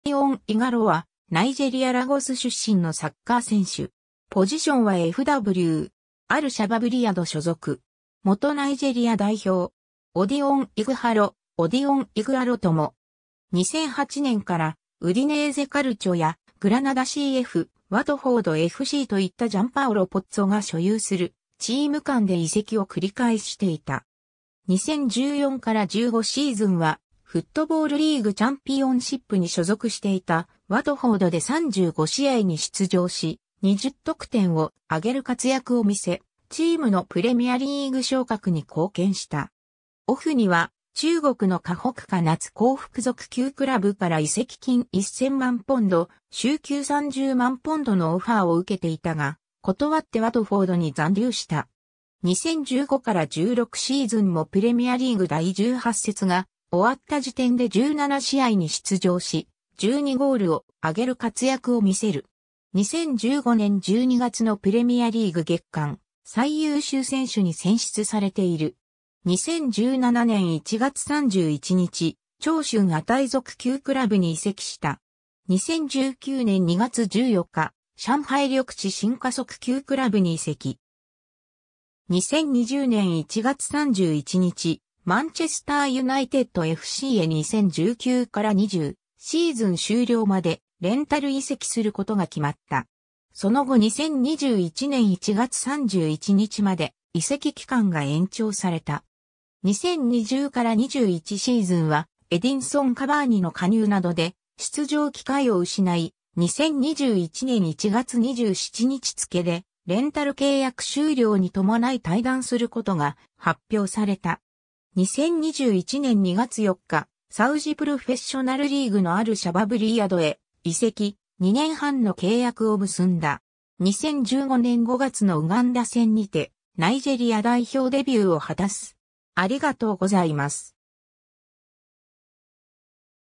オディオン・イガロ
ウディネーゼ・カルチョの選手
オディオン・イガロ（Odion Ighalo, 1989年6月16日 - ）は、ナイジェリア・ラゴス出身のサッカー選手。
アル・シャバブ・リヤド所属。
元ナイジェリア代表。
オディオン・イグハロ、オディオン・イグアロとも。
 2008年からウディネーゼ・カルチョやグラナダCF、ワトフォードFCといったジャンパオロ・ポッツォが所有するチーム間で移籍を繰り返していた。
 2014-15シーズンはフットボールリーグ・チャンピオンシップに所属していたワトフォードで35試合に出場し、20得点を挙げる活躍を見せ、チームのプレミアリーグ昇格に貢献した。
オフには中国の河北華夏幸福足球倶楽部から移籍金1000万ポンド（約16億9000万円）、週給30万ポンド（約5085万円）のオファーを受けていたが、断ってワトフォードに残留した。
 2015-16シーズンもプレミアリーグ第18節が終わった時点で17試合に出場し、12ゴールを挙げる活躍を見せる。
2015年12月のプレミアリーグ月間最優秀選手に選出されている。
 2017年1月31日、長春亜泰足球倶楽部に移籍した。
 2019年2月14日、上海緑地申花足球倶楽部に移籍。
 2020年1月31日、マンチェスター・ユナイテッドFCへ2019-20シーズン終了までレンタル移籍することが決まった。
その後2021年1月31日まで移籍期間が延長された。
2020-21シーズンはエディンソン・カバーニの加入などで出場機会を失い、2021年1月27日付でレンタル契約終了に伴い退団することが発表された。
 2021年2月4日、サウジ・プロフェッショナルリーグのアル・シャバブ・リヤドへ移籍、2年半の契約を結んだ。
 2015年5月のウガンダ戦にてナイジェリア代表デビューを果たす。